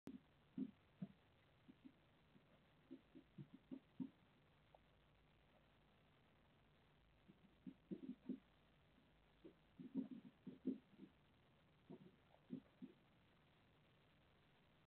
Bullfinch slapstick
Hilarious movement of two bullfinches (Pyrrhula pyrrhula) on the window feeder: The male slides towards the female, gets threatened, tries to distance itself, but only manages to slide further in. Where he is then actually scared of by her, and the attempt to return to the other side of the feeder is also a hopeless effort.